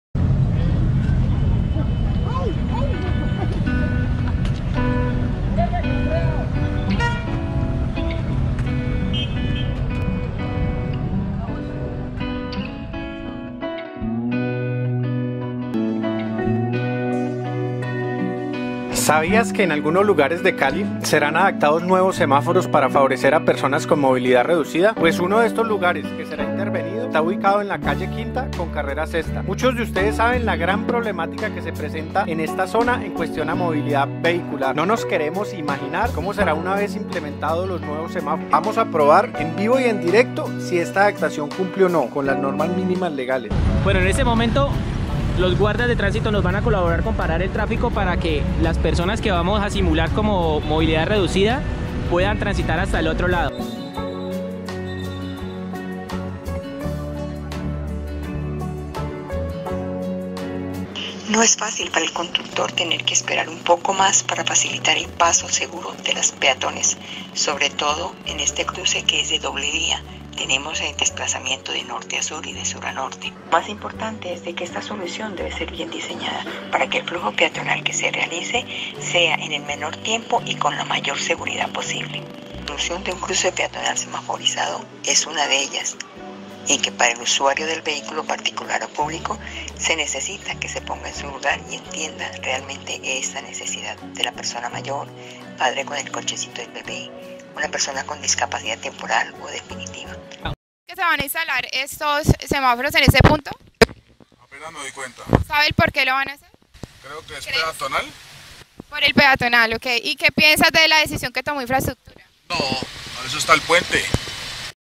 EL CAOS VEHICULAR DE LA CALLE 5
CALI INFORMA, advirtió del caos que se formaría en la calle 5 con cra 6 por la.puesta en marcha de los semaforos para personas con movilidad reducida.